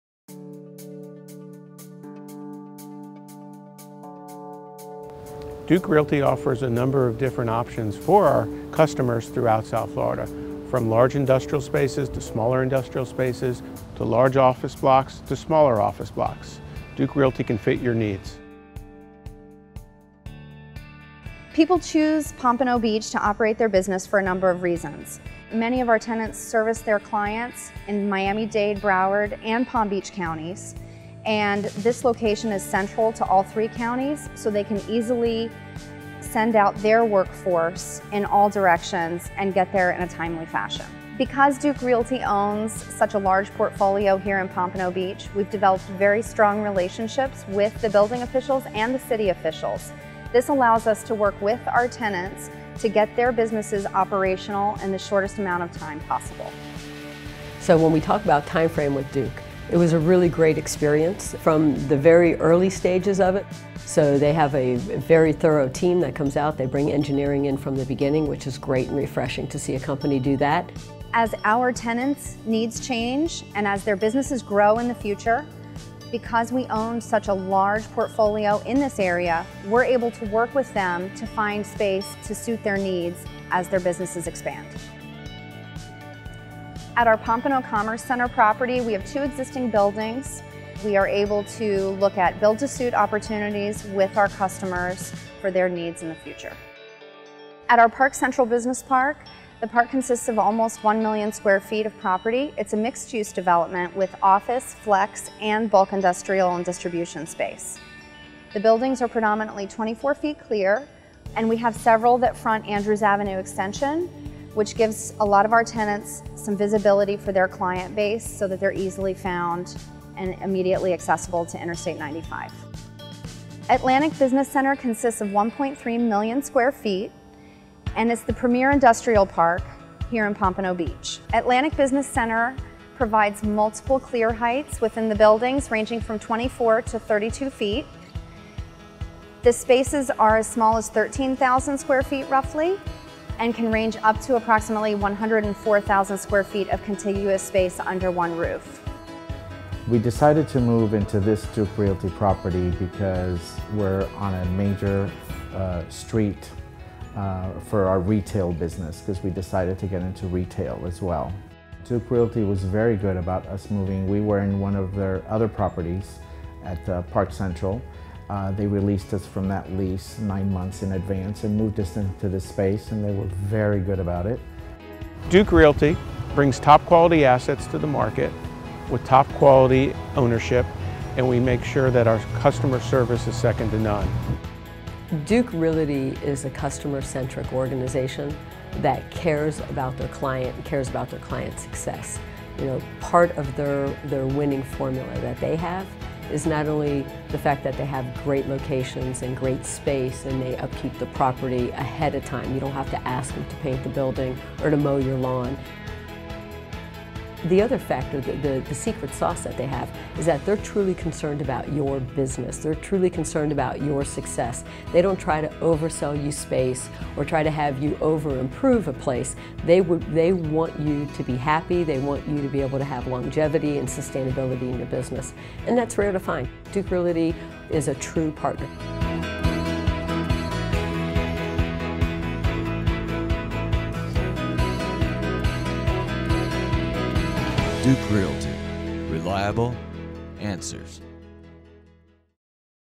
Pompano Beach Portfolio
Duke Realty’s Pompano Beach portfolio is approximately three million square feet of high-quality, warehouse/distribution space. Our development parks, Pompano Commerce Center, Park Central Business Park and Atlantic Business Center, offer nearby amenities, easy access to all points of the tri-county area, and close proximity to major airports.